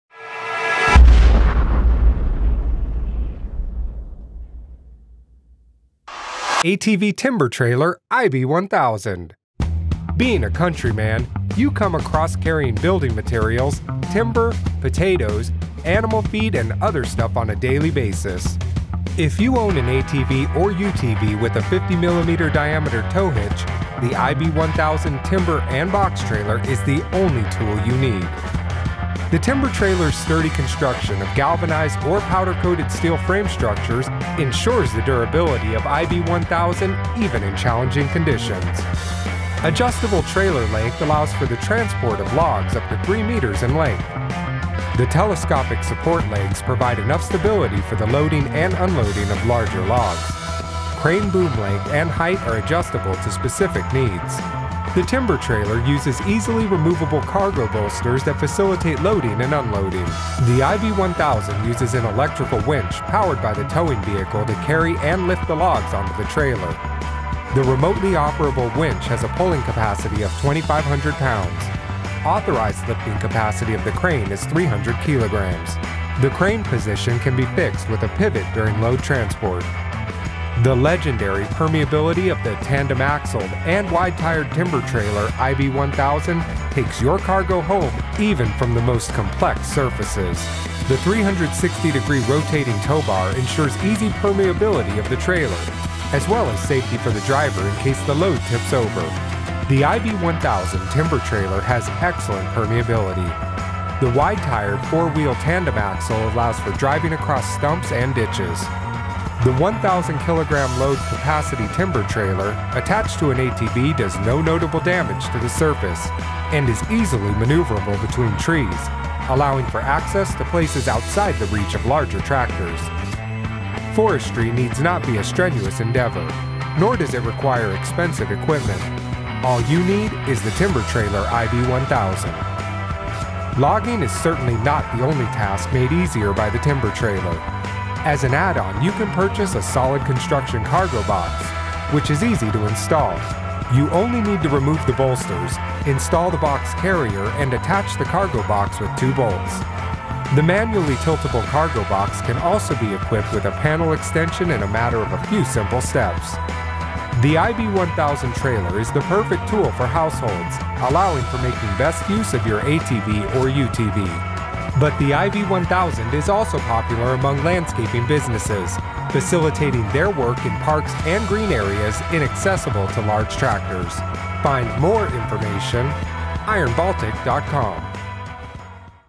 ATV TIMBER TRAILER "IB-1000" ( ATV & UTV Accessories )
Strong, steel frame structure
Galvanized or powder coated surfaces
360° rotating towbar
Support legs
Rotating and removable bolsters
Adjustable trailer length / adjustable loadbed lenght
Max possible length of carriage is 3 meter
Adjustable crane support legs
Adjustable crane boom length
Adjustable crane boom height
Transport position of the crane can be fixed with a pivot
Electric winch with wireless remote control. Pulling force 2500lbs
Permitted lift capacity max 300kg
Four wheel tandem axle
Trailer load capacity 1000kg
Tiltable cargo box
Removable end panels and panel extensions on cargo box